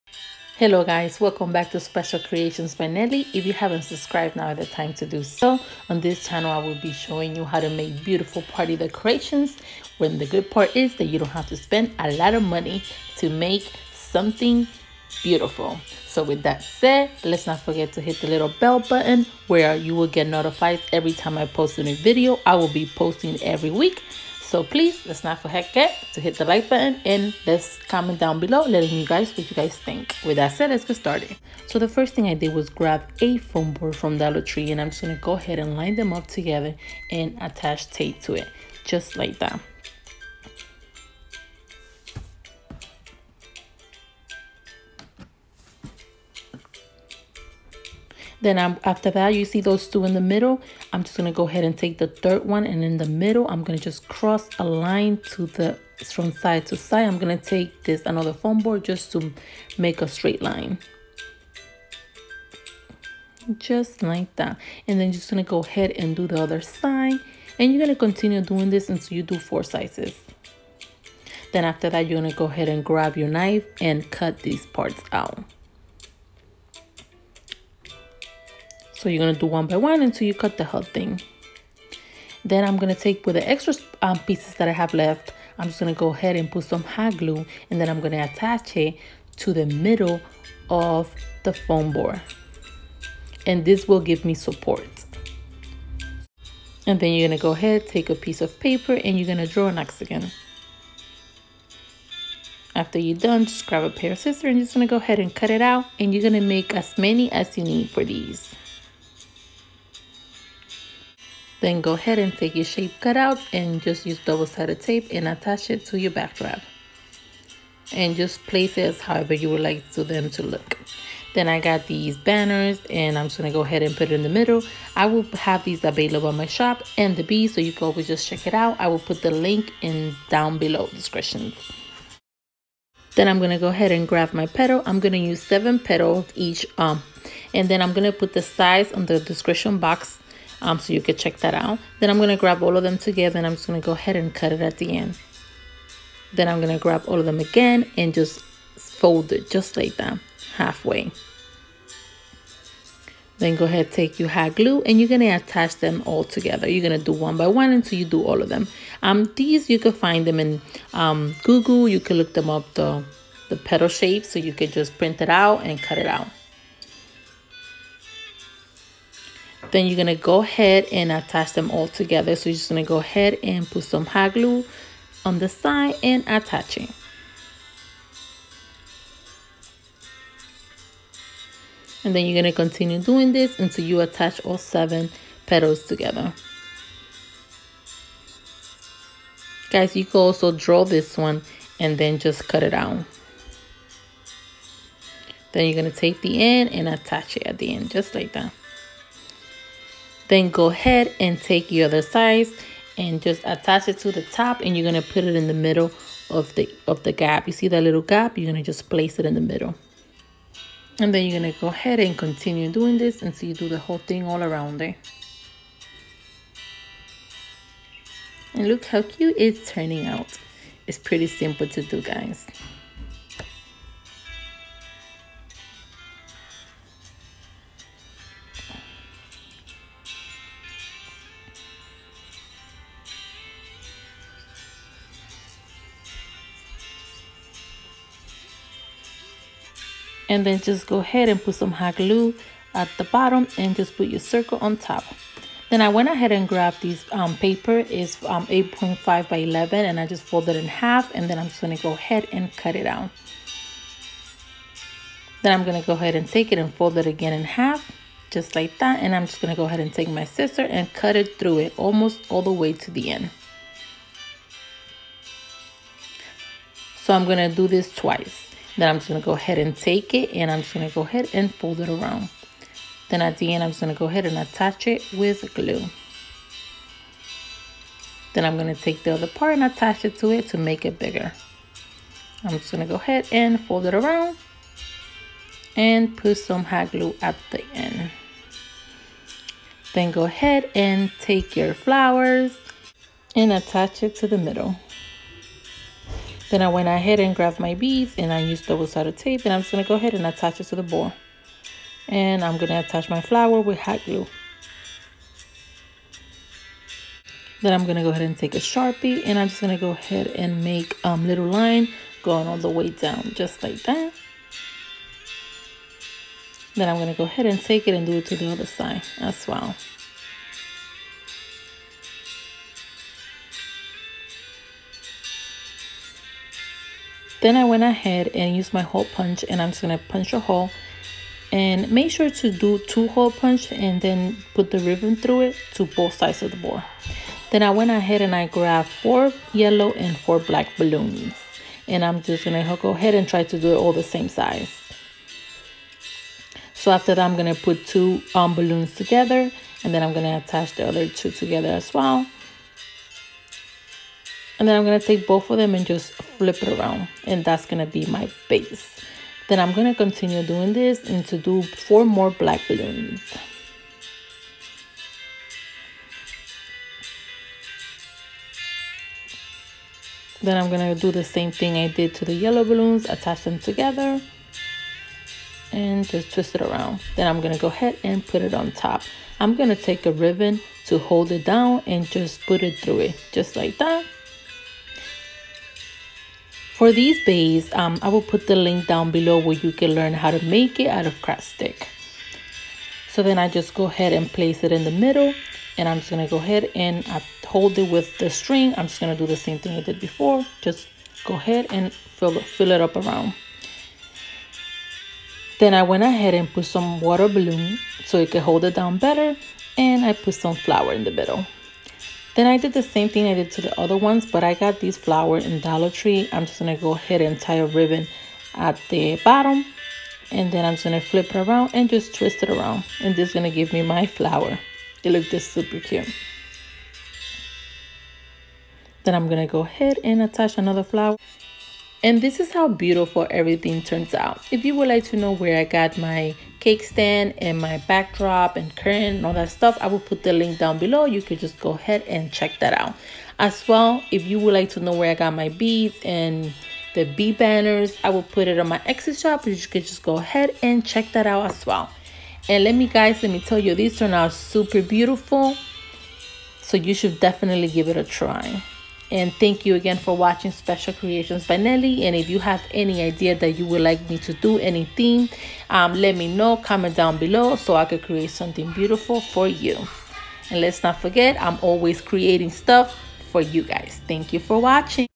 Bumblebee Party DIY/ Bee Birthday Party/DIY Bee Baby Shower /DIY Gender Reveal ?What Would It Bee?
Hello guys!! watch me create the sweetest  Bumble bee party decorations that can be use for Bumble bee Baby Shower,Mommy to bee, theme showers beehive baby shower, Bee Birthday party, Bee Day, beehive gender reveal. these decorations can be use for all these party theme.

BALLOONS PUMP

link to created the base for the balloons stand

Link for the Bee cut out, bee favor box, and banner.

flower petal size
big petal :9.027 in by 3.282
medium petal :7.693 in by 2.799